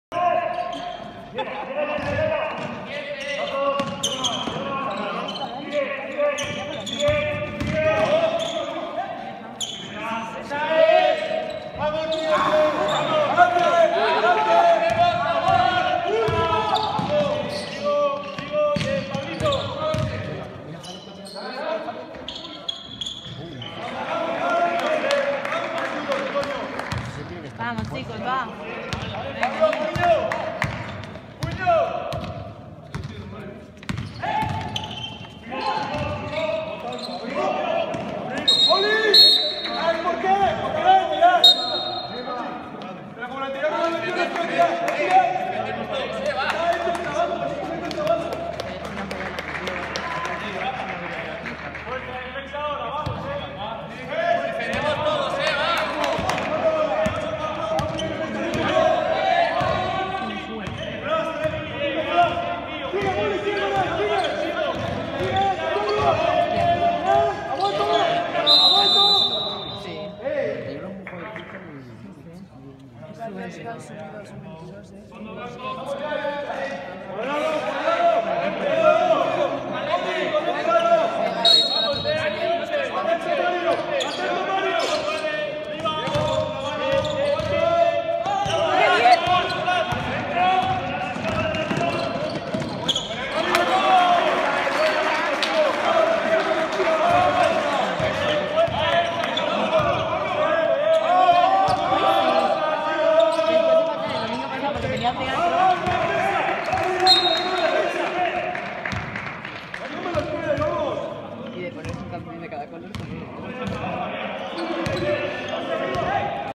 VILLANUEVA DE LA CAÑADA-VILLA DE VALDEMORO; 1ª Autonómica Masculina 'PLATA' 2019-2020 (3/6)
8 de Marzo de 2020
Pvo.Mpal.Santiago Apóstol
Villanueva de la Cañada